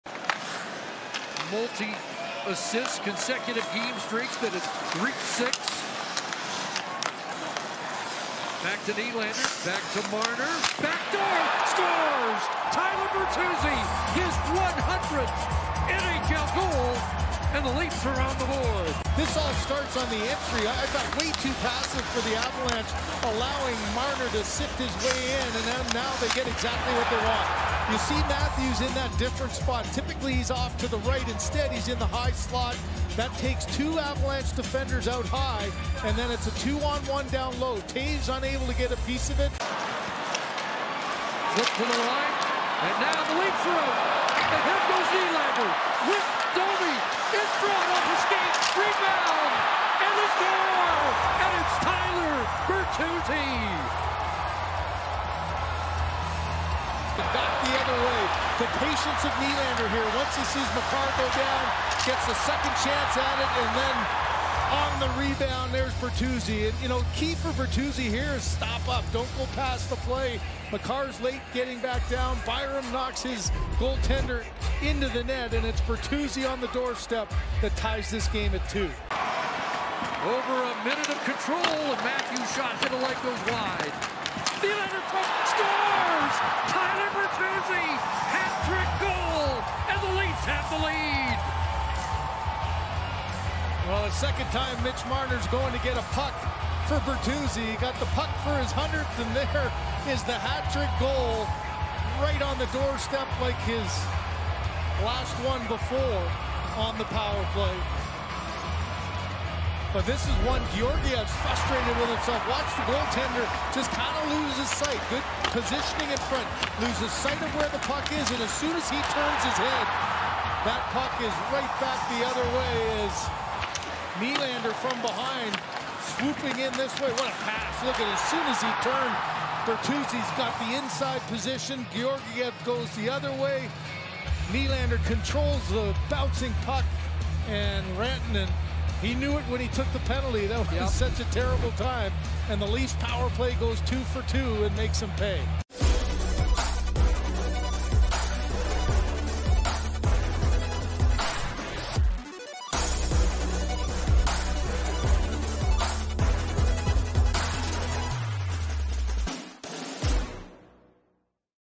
Tyler Bertuzzi Makes Maple Leafs History With A Birthday Hat Trick
Watch as Toronto Maple Leafs forward Tyler Bertuzzi celebrates his 29th birthday in style, completing his hat trick with a go-ahead goal late in the third against the Colorado Avalanche to become the first Leaf ever to record a birthday hatty.